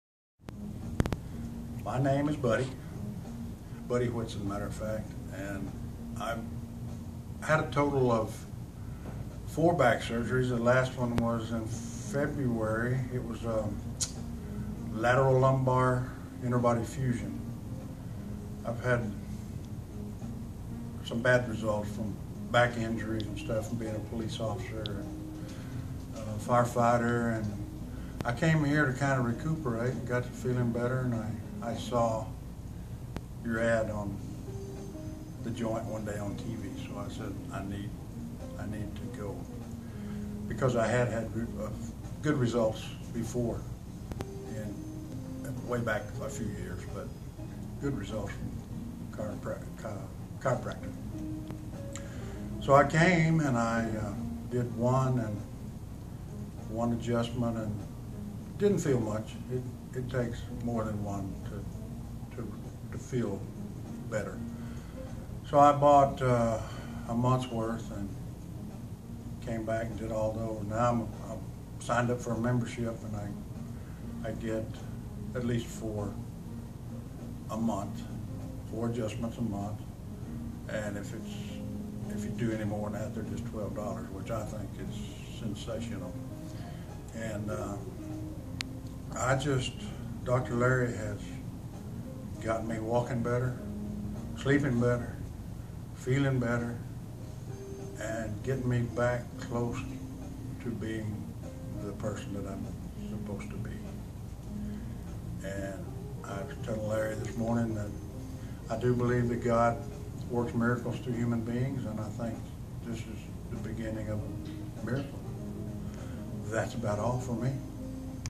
Buddy testimonial
4 lumbar surgery and now after a month of care buddy is doing great